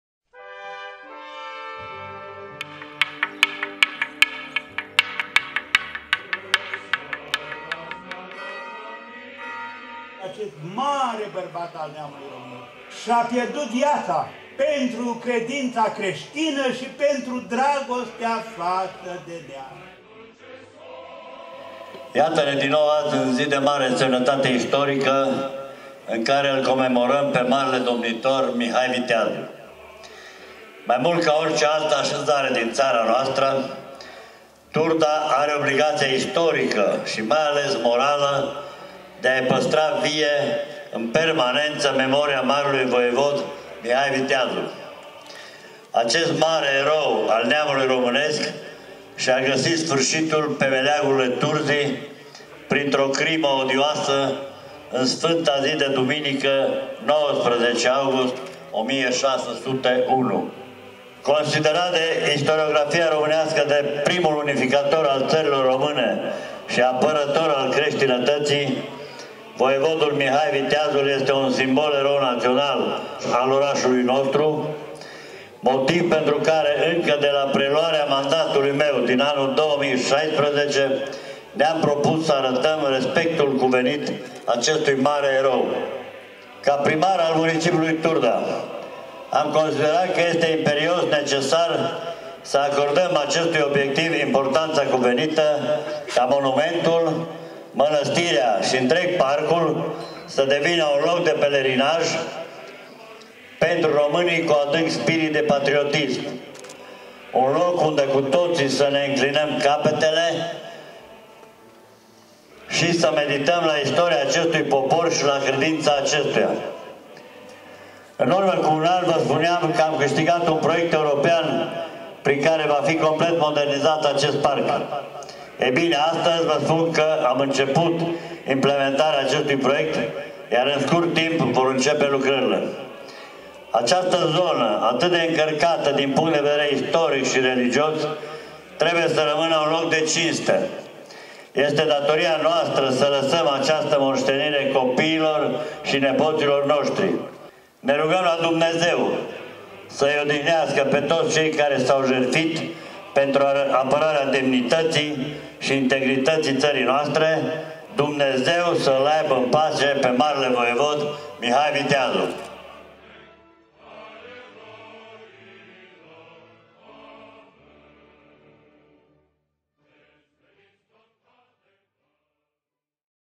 Comemorarea marelui Voievod Mihai Viteazul
Mai mult ca orice altă așezare din țara noastră, Turda are obligația istorică și mai ales morală, de a-i păstra vie, în permanență, memoria Marelui Voievod Mihai Viteazul. Acest mare erou al neamului românesc și-a găsit sfârșitul pe meleagurile Turzii, printr-o crimă odioasă, în Sfânta zi de Duminică, 19 august 1601.

Considerat de istoriografia românească drept primul unificator al Țărilor Române și apărător al creștinității, Voievodul Mihai Viteazul este un simbol erou național al orașului nostru. Dumnezeu să îi odihnească pe toți cei care s-au jertfit pentru apărarea demnității și integrității țării noastre.